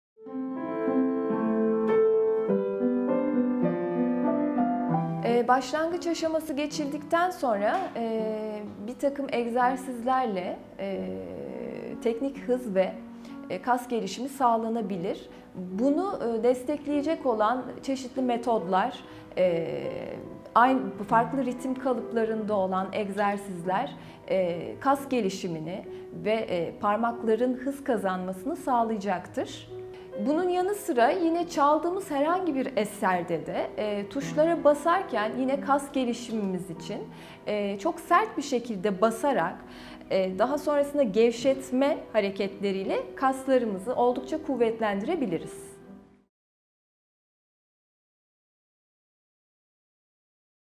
Piyano Çalmayı Kolaylaştıracak Egzersizler Nelerdir?
Başlangıç aşaması geçildikten sonra bir takım egzersizlerle teknik hız ve kas gelişimi sağlanabilir. Bunu destekleyecek olan çeşitli metotlar, farklı ritim kalıplarında olan egzersizler, kas gelişimini ve parmakların hız kazanmasını sağlayacaktır.

Bunun yanı sıra çaldığımız herhangi bir eserde, tuşlara çok sert biçimde basarak daha sonra gevşetme hareketleriyle kaslarımızı oldukça kuvvetlendirilebiliriz.